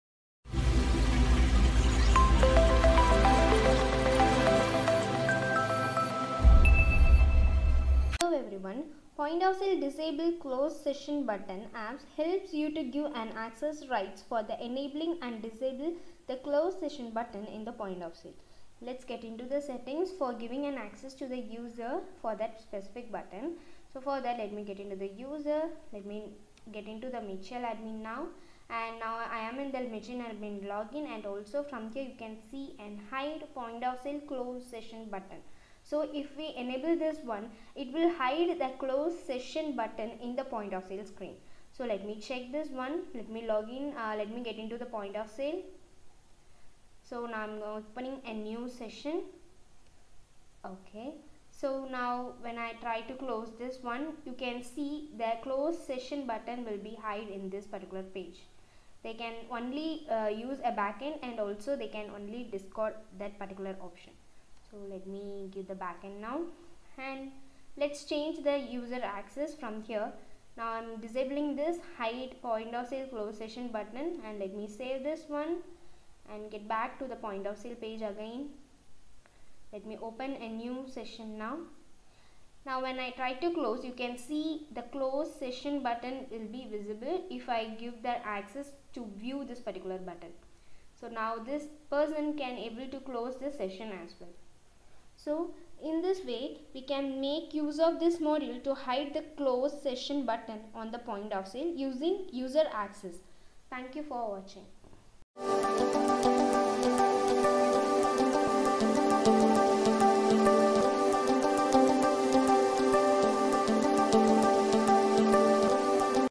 Disable POS Close Session Button With Access Control in Odoo
POS- Point Of Sales Disable Close Session Button Odoo App helps users to give access rights to enable/disable the 'Hide POS Close Session Button' checkbox from user configuration. If the user ticked then the user will have deactivated the closed session otherwise activate the closed session and the user can close the session.

Features of this Odoo App:

-- Enable the 'Hide POS Close Session Button' Checkbox
-- Disable the 'Hide POS Close Session Button' Checkbox

** Related Odoo Apps

1. POS Order Notes in odoo
2. Odoo Edit/Modify POS Receipt App
3. Odoo POS Customer Pricelist